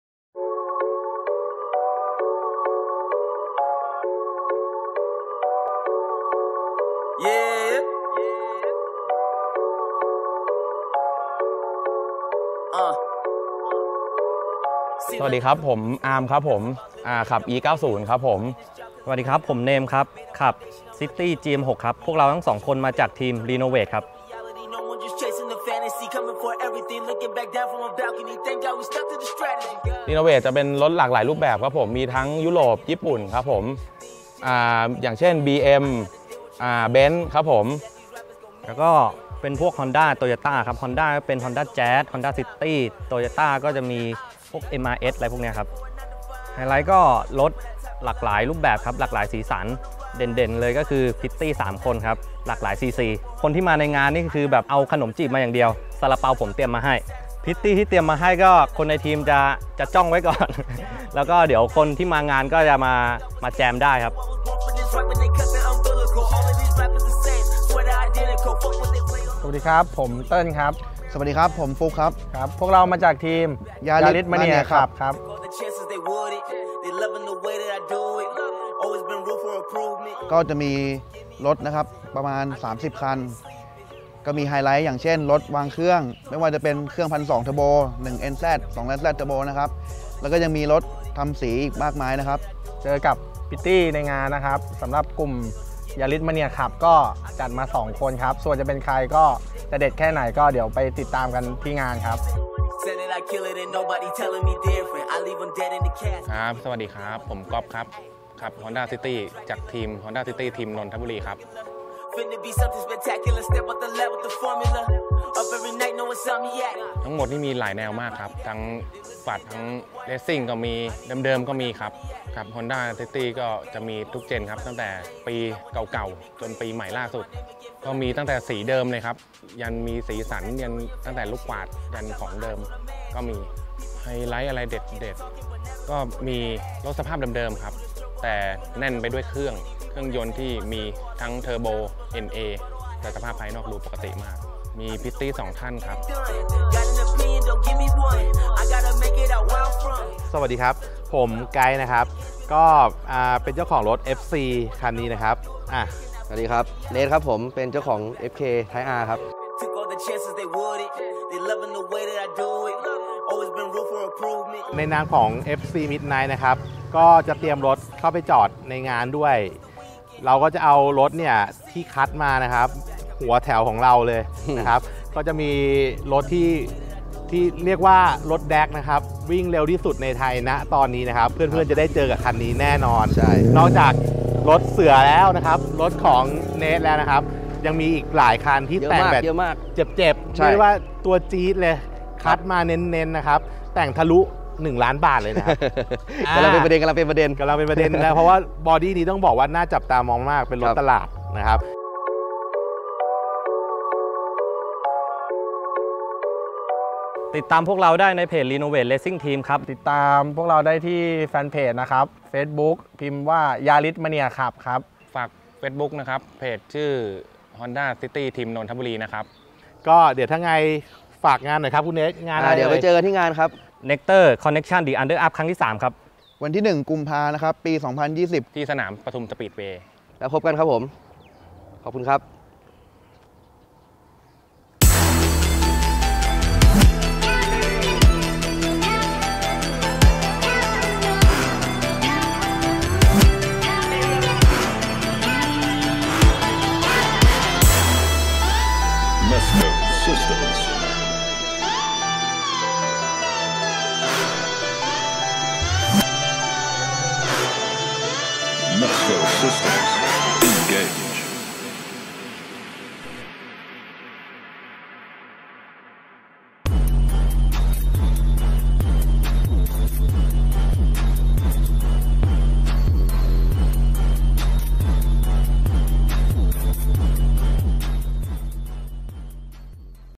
มาดูความพร้อมของ 4 ทีม ก่อนไปลุยงาน”Nexzter Connection The Underupx3”
เทศกาลประจำปีที่ขาซิ่ง รอคอยกับความยิ่งใหญ่และมันส์กว่าเดิม. ในคอนเซปต์ Neon Light Party ใน “Nexzter Connection The Under Up X 3”

มหกรรมการรวมพล รถแต่งแห่งชาติ แน่นทุกกิจกรรม Drift-Drag-Show-Party

ระเบิดความมันส์ เที่ยงวัน - เที่ยงคืน
วันเสาร์ที่ 1 กุมภาพันธ์ 63 ณ สนามปทุมธานี  สปีดเวย์
บัตรเข้างานราคา 100.-บาท

Facebook Page  Boxzaracing และ Connection Thailand.